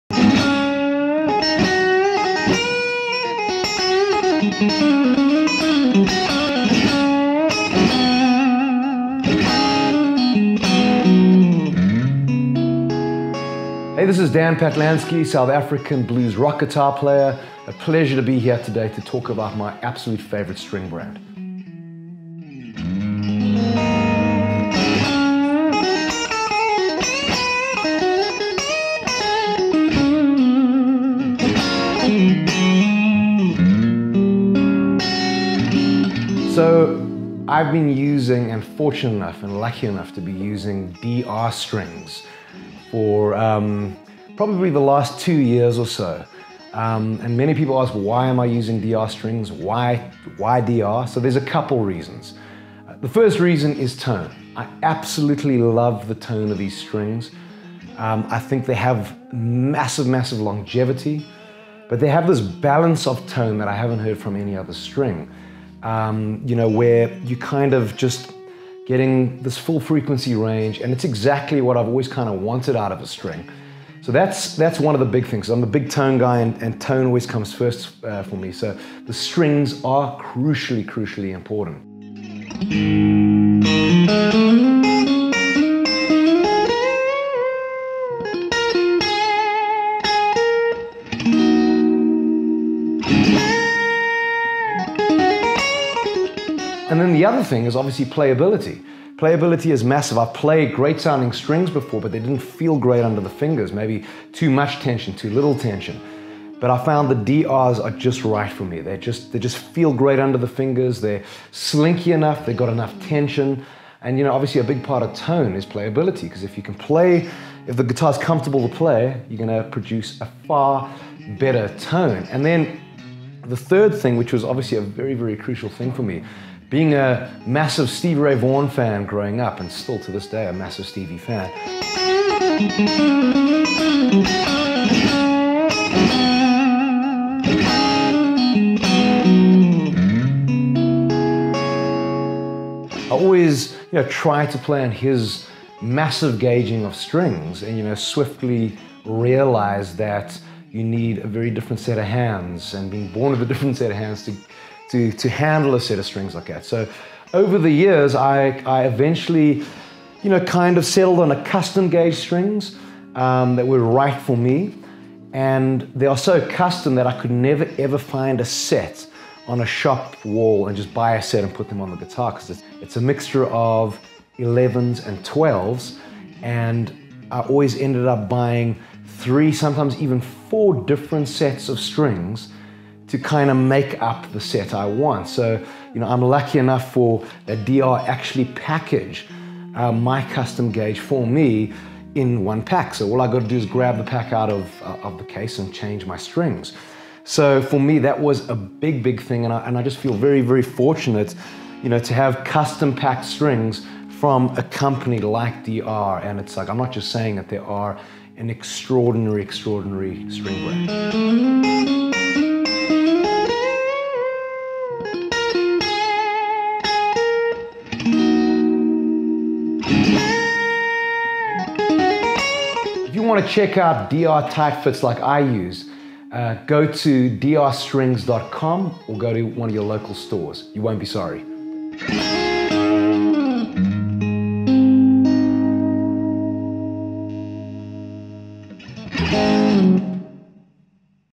Dan Patlansky & DR Strings
Dan Patlansky is just an incredible guitarist that without a doubt gets insane tone from his gear!

His strings of choice are DR STRINGS and here's Dan explaining why he chooses DR Strings above everything else!

To check out the wide range of DR Strings that Marshall Music stocks, just follow this or to visit DR Strings's website just go